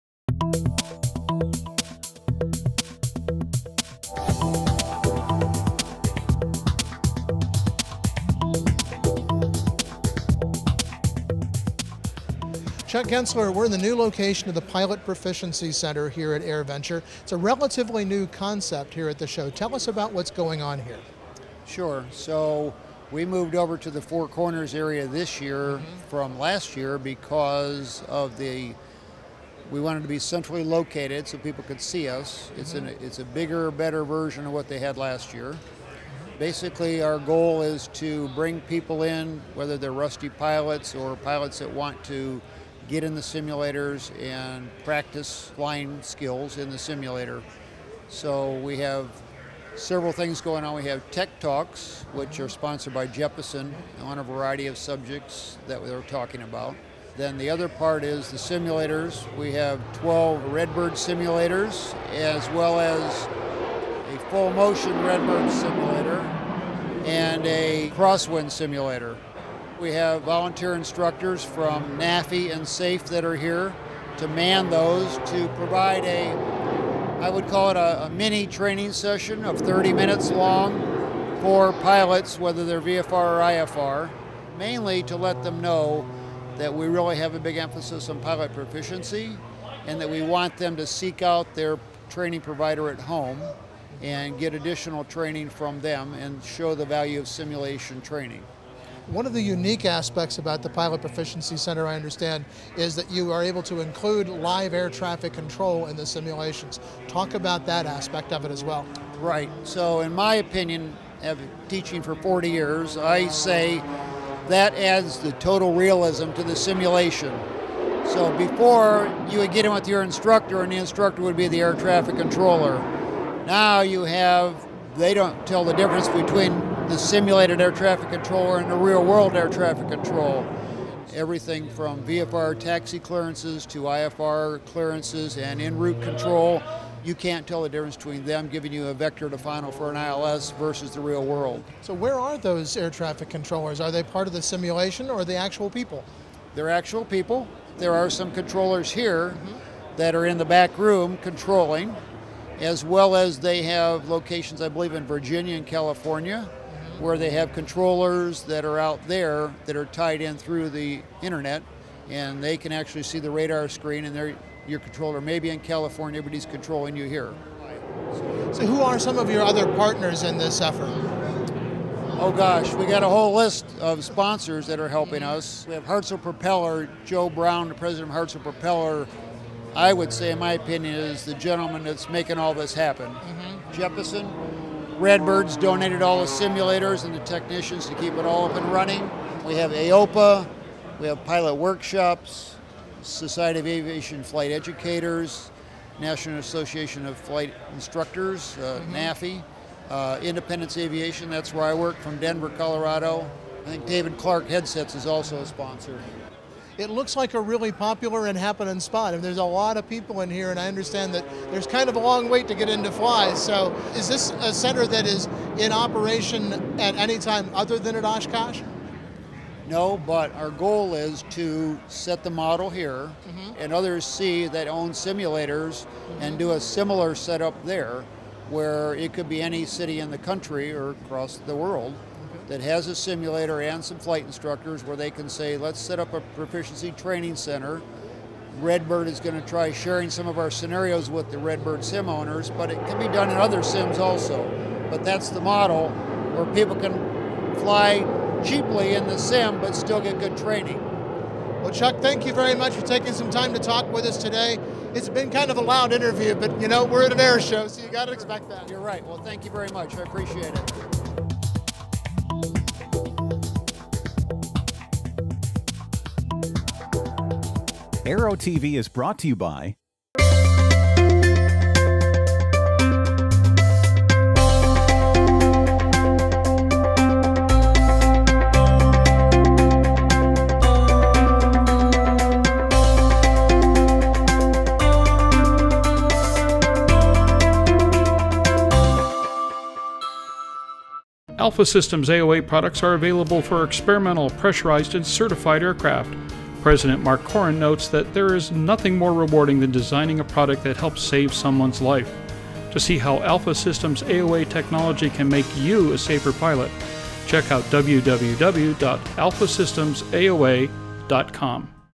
Aero-TV: Maintaining Your Flying Skills - EAA 2016's Pilot Proficiency Center
EAA’s Pilot Proficiency Center Is Designed to Encourage Proficiency and Provide Actual Training…

While at EAA AirVenture 2016, ANN News Editor, Tom Patton, stopped by the Pilot Proficiency Center in its new location at what was known as the, EAA Four Corners. This new central location brings to light EAA’s position that pilot proficiency is key to aviation safety.

Tom met up with Chuck Gensler, who is the Operations Manager for EAA’s Pilot Proficiency Center. Gensler pointed out that the purpose of the Pilot Proficiency Center was not only to promote the concept that proficiency is key to safety, but also to put it into practice at EAA AirVenture.

Throughout the week, the center offered forums and training programs, as well as 14 simulators, which allowed the volunteer instructors to really perform proficiency training. Hundreds of pilots actually left the Pilot Proficiency Center feeling better about their skills than when they walked in the door.

Another fascinating feature of the training received at the Pilot Proficiency Center was that air traffic control in the simulators was handled like the real deal. Actual ATC controllers were located in a separate area of the center while others were linked from across the country.

It’s a great video for seeing how modern training can be performed, and the role that maintaining pilot proficiency can play in overall aviation safety. Be sure you visit the Pilot Proficiency Center if you attend AirVenture 2017!